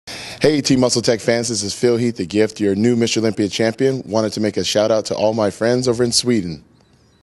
Greeting from Phil Heath
We got a greeting from Phil Heath to all swedish fans!